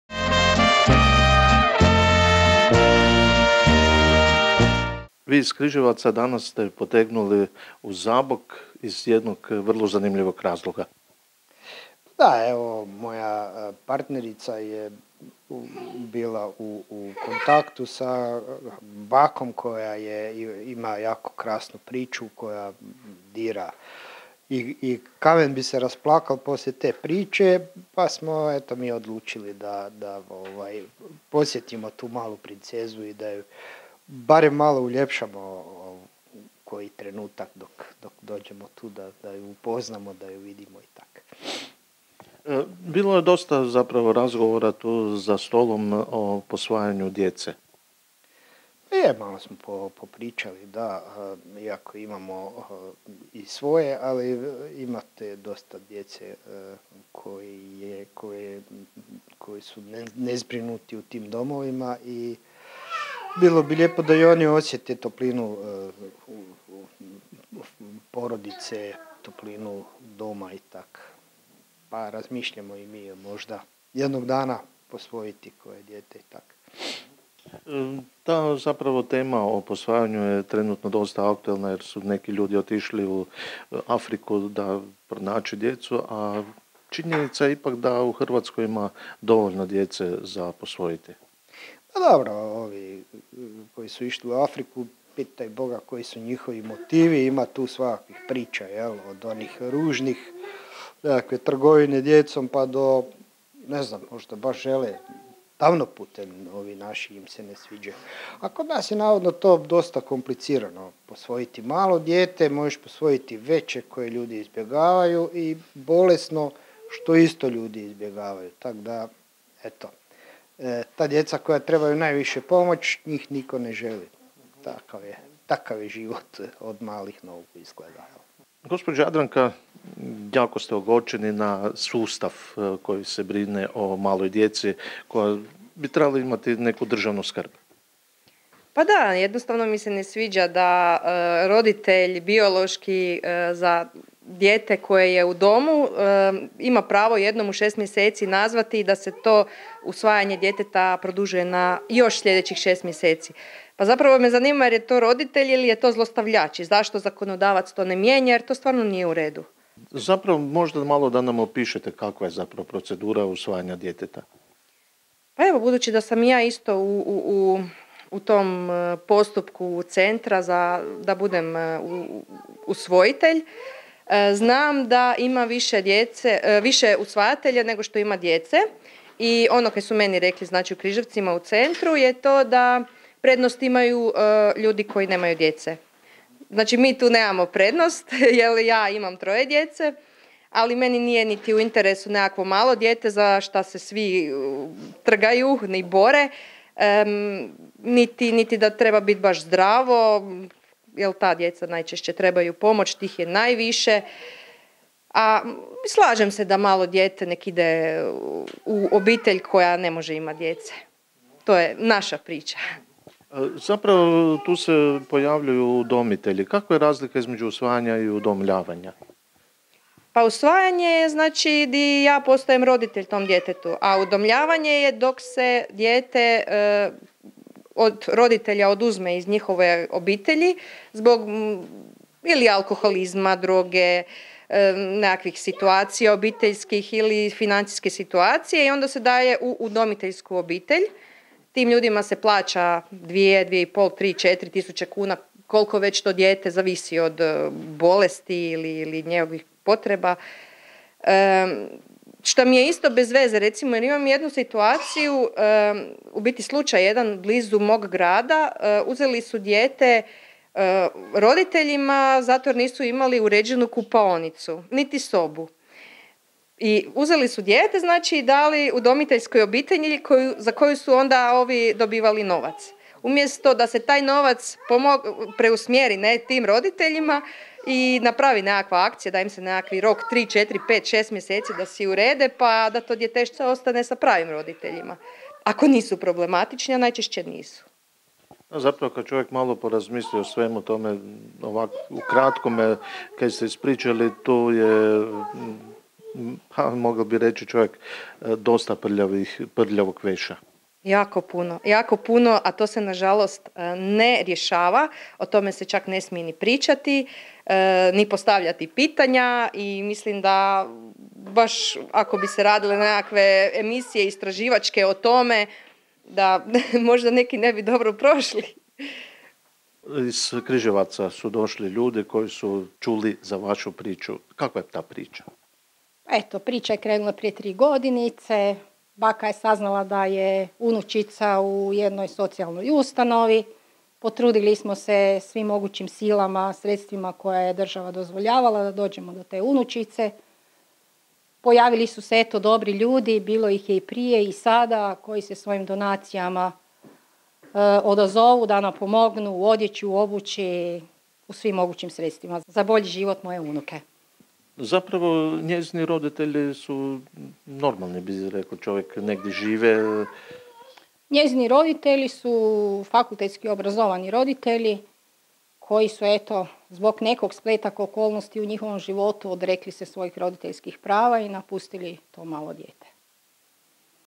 Zagorje moje 26.01.23. - Posvajanje djece
V prilogu morete videti kak su ljudi s Križevcof došli v Zabok i donesli donaciju za djevojčicu koju su roditelji napustili te o njoj brine baka a usput smo se i pospominali o posvajanju djece koje je pri nas zakonski loše riješeno. budite z nami lučite nas.